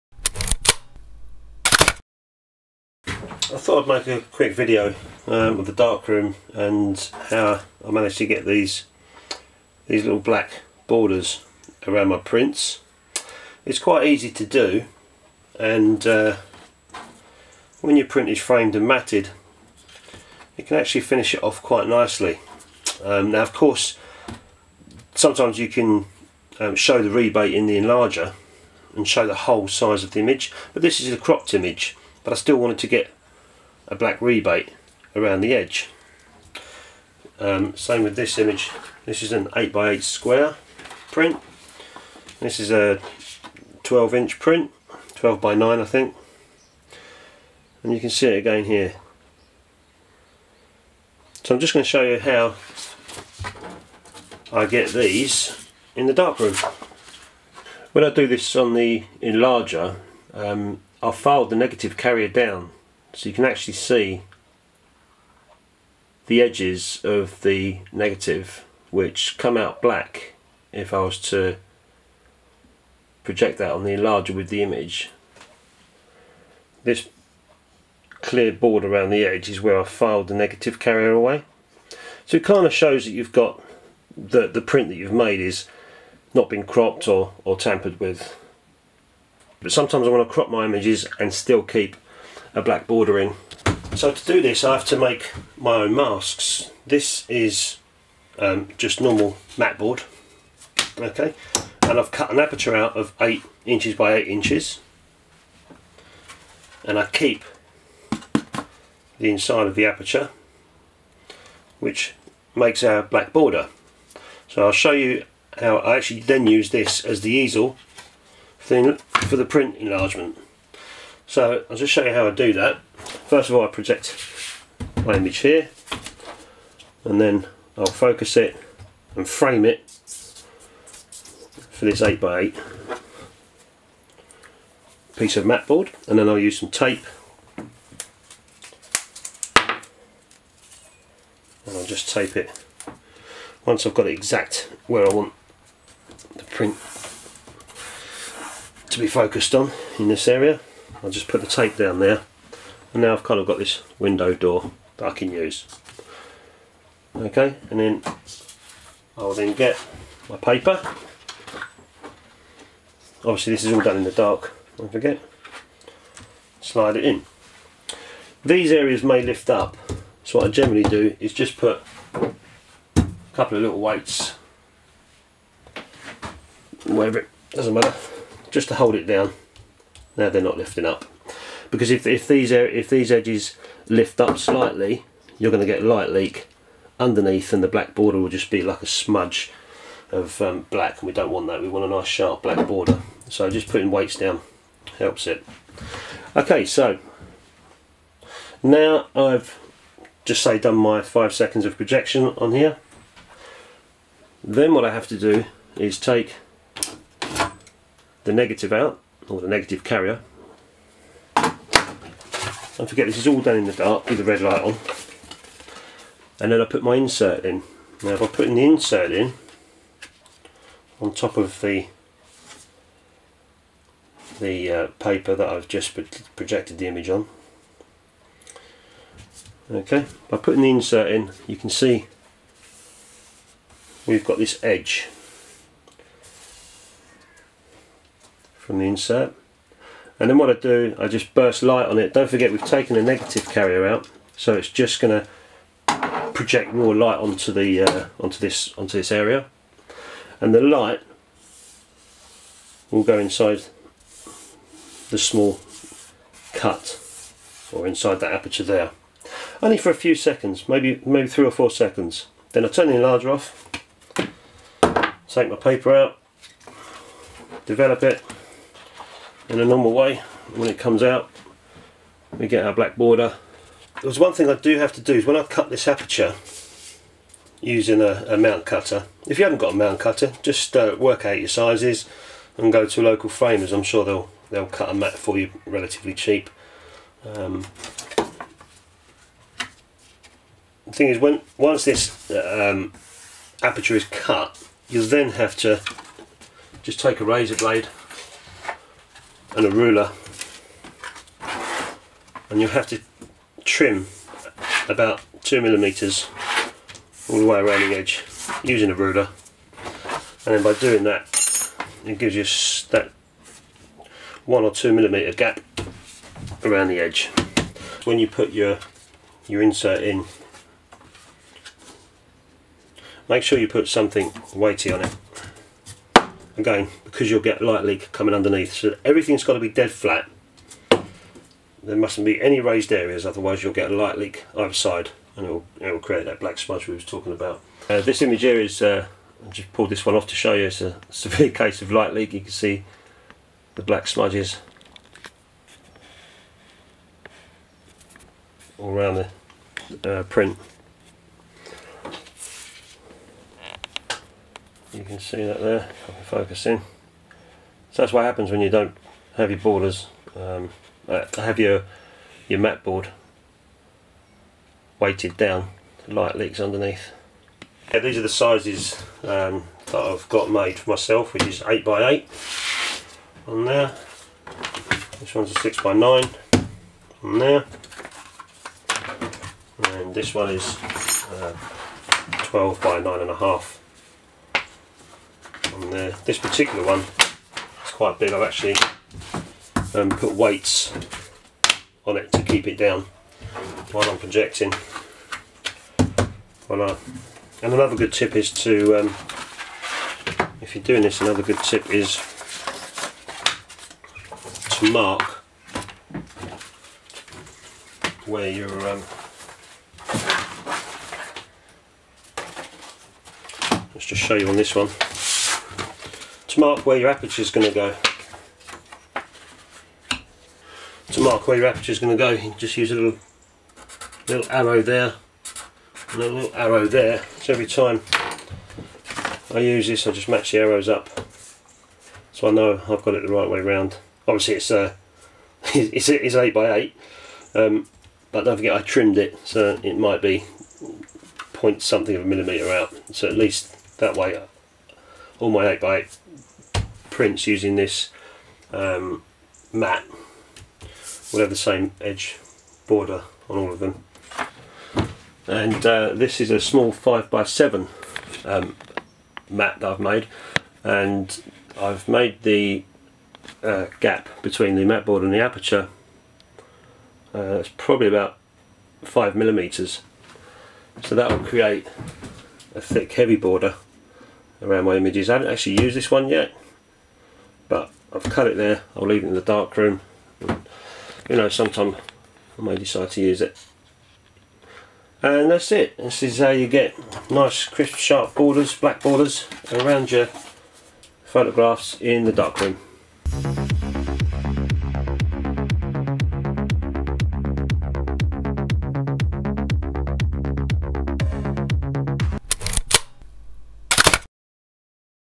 Darkroom Photography Process - BLACK BORDERS
This is how I make black borders in the darkroom on my cropped prints. Very simple to do and also looks good.